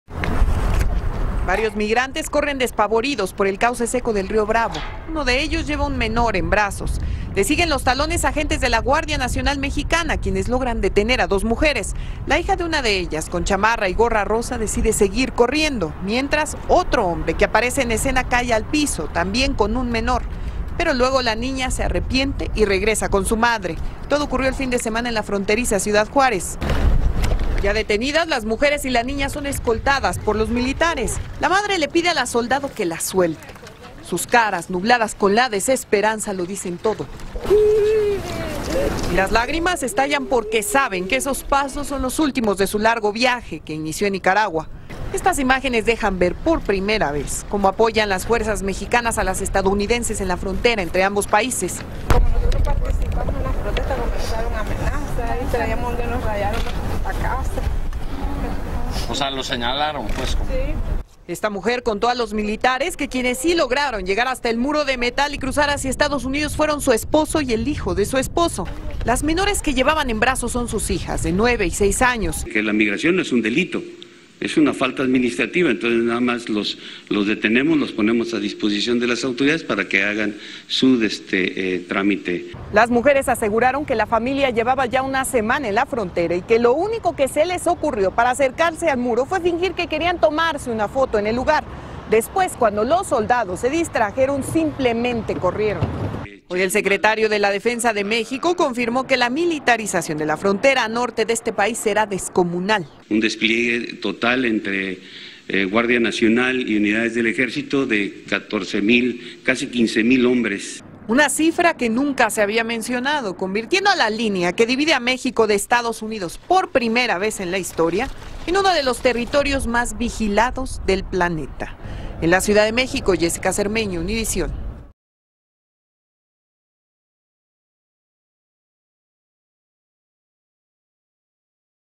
México destinará casi 15 mil uniformados para custodiar la frontera con Estados Unidos
Así lo informó el secretario de defensa mexicano, Luis Cresencio Sandoval, quien aseguró que la inmigración no es un delito sino una falta administrativa, por lo que los centroamericanos que detienen son puestos a disposición de las autoridades. Los oficiales de la Guardia Nacional y el Ejército intentarán detener el flujo de migrantes en territorio fronterizo.